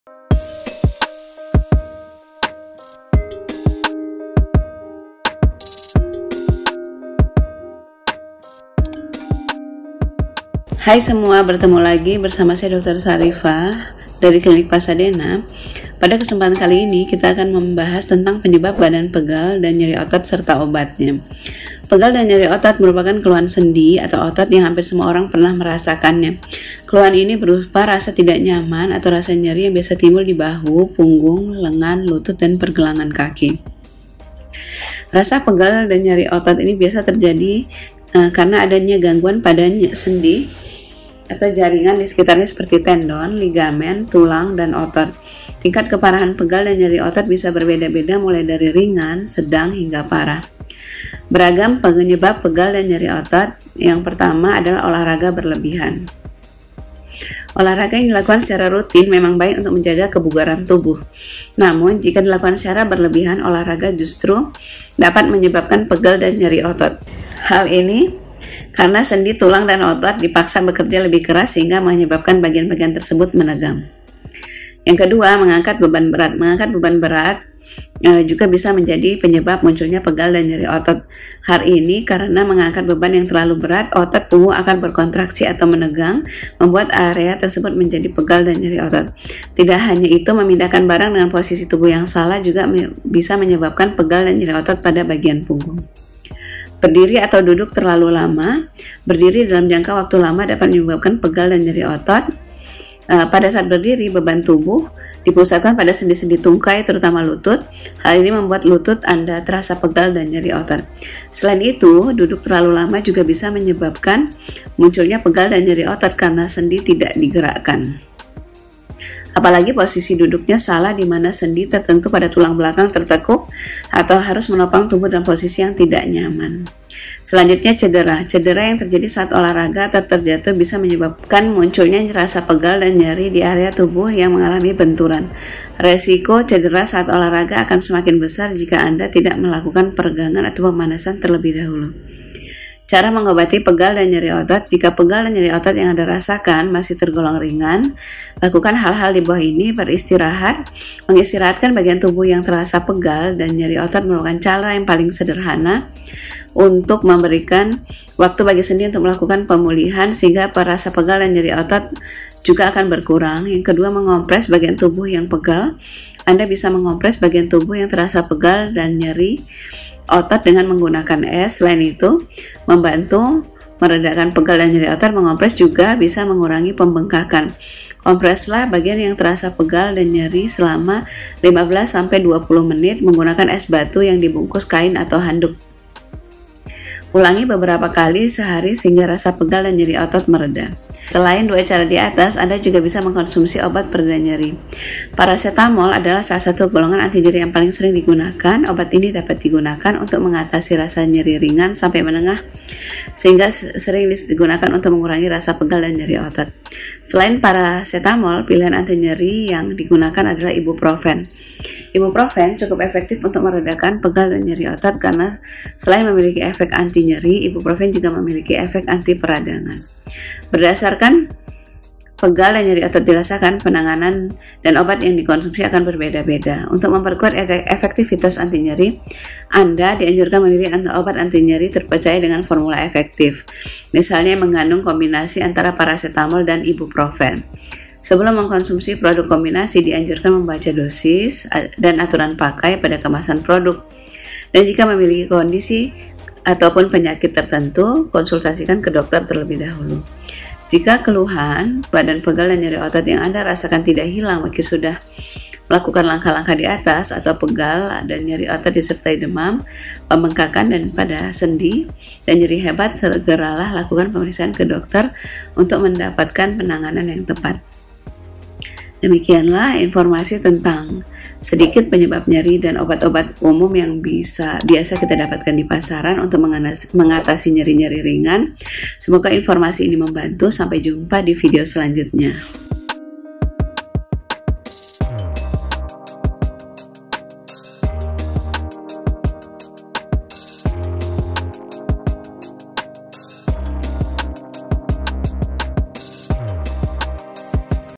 BADAN TERASA PEGAL DAN SAKIT SEMUA
badan terasa pegal dan sakit semua, penyebab seperti mengangkat berat yang berlebih salah posisi ketika menggerakkan badan dan lainnya
bisa juga menyebabkan pegal linu seluruh badan dan menyebabkan nyeri otot

untuk informasi selengkapnya ikuti videonya bersama dokter Syarifah Azizah dari klinik Pasadena image picture thanks